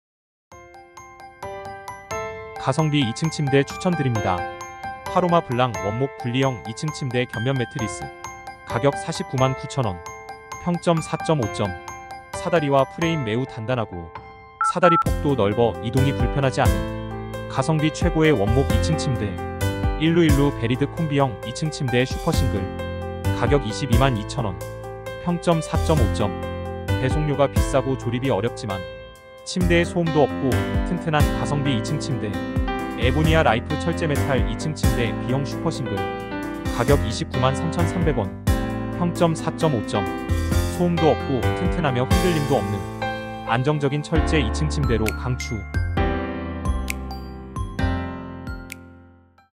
23년 8월 가성비 이층침대 추천 BEST 3
지금사면 훨씬 싸게 구매 가능!

📌 가성비 이층침대 추천 링크 :

1. 파로마 블랑 원목 분리형 이층침대 경사형_견면매트리스 포함

2. 일루일루 베리드 콤비형 이층침대 슈퍼싱글+싱글

3. 에보니아 라이프 철재메탈 이층침대 B형 슈퍼싱글 매트선택

수십가지 이층침대 비교 후 추천드립니다!

튼튼하고 흔들림 없고 소음이 없는

가격대비 훌륭한 품질을 보여주는

이츰침대 제품들을 선정했습니다~

여기서 고르시면 후회 안하십니다!

구경이라도 한번씩 해보시면 좋을 거 같습니다~

※ 업체로부터 광고비를 지급받지 않고 직접 선정한 순위입니다.

※ 링크는 스마트폰 이용을 권장드립니다.

※ 가격은 실시간으로 변동될 수 있습니다.